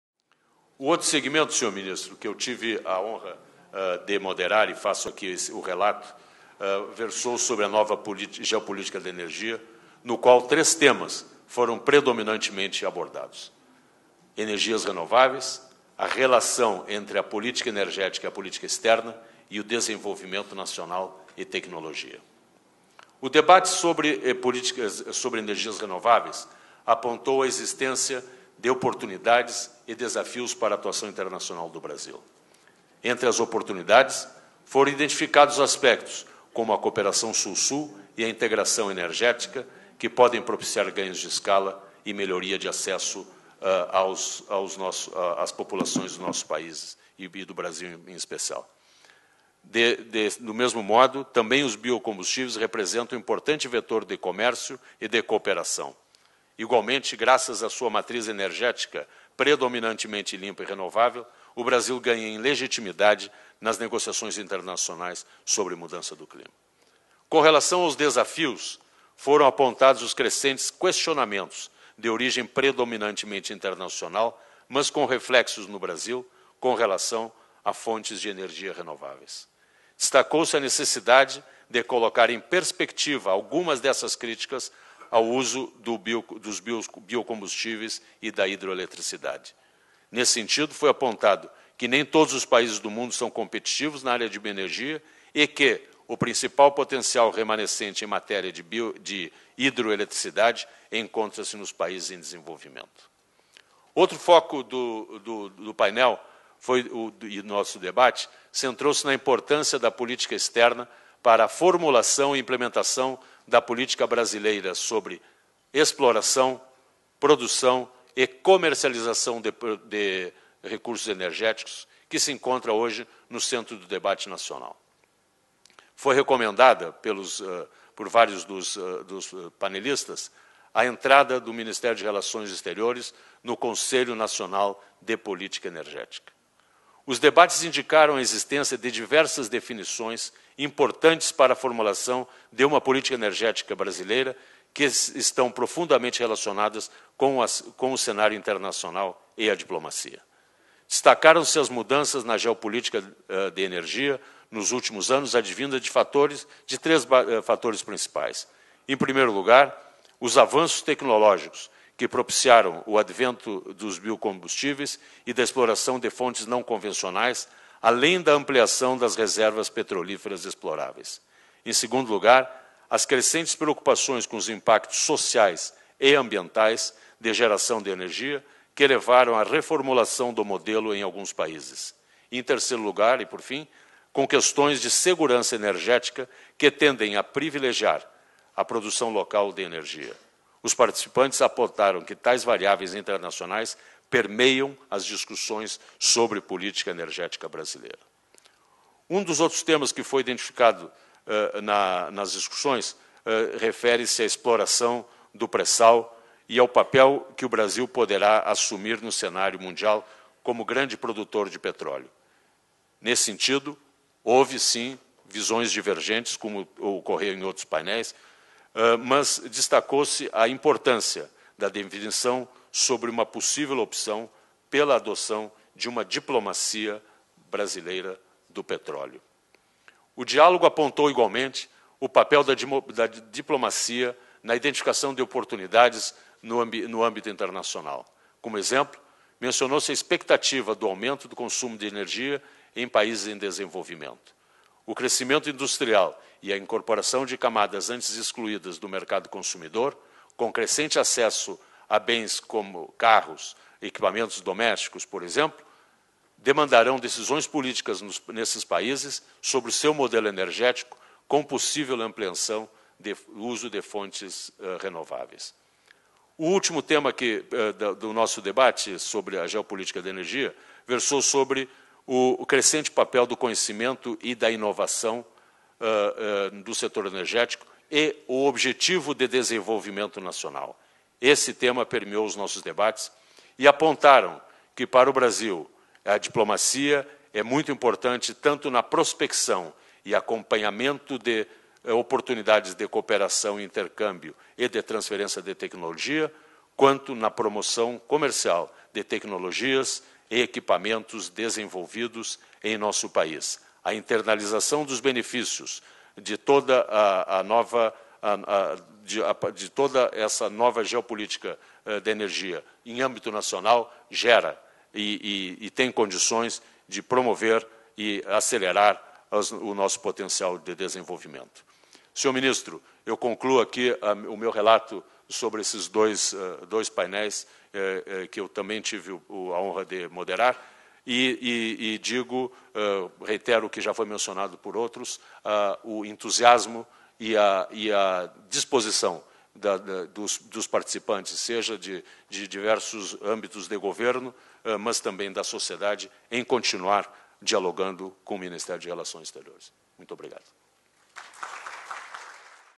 Relatório do painel "A nova geopolítica da energia".
O Embaixador José Antonio Marcondes de Carvalho, Subsecretário-Geral de Meio Ambiente, Energia e Ciência e Tecnologia,  apresenta o relatório final do painel "A Nova geopolítica da energia", realizado no dia 21 de março de 2014, em Brasília.